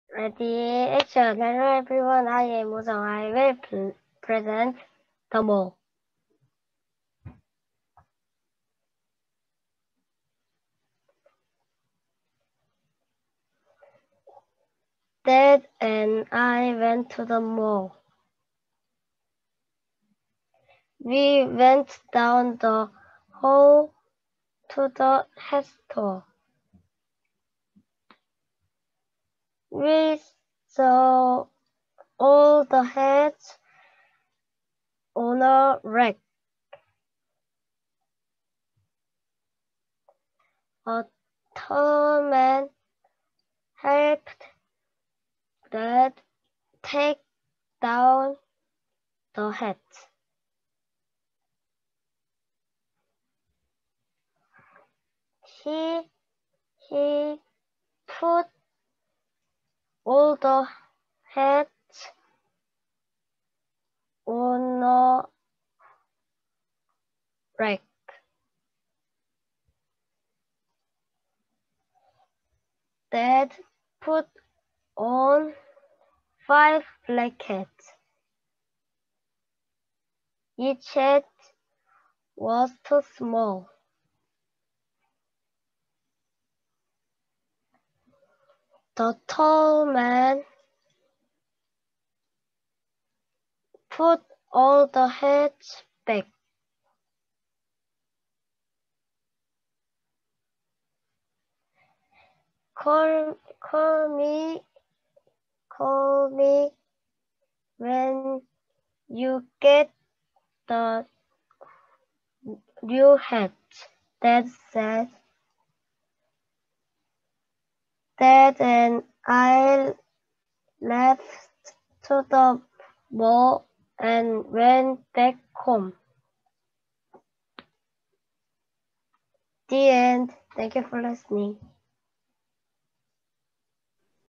차일드유 월드컵 211 EC
비대면 수업을 하면서 두번째 온라인 발표회를 진행했어요~
직접 zoom 을 통해서 화면 건너에 있는 상대에게 발표하는 새로운 경험이었습니다.
아이들 모두 오랜만에 떨리는 순간 느끼고 잘 발표하여 뿌듯하다고 하네요 ^^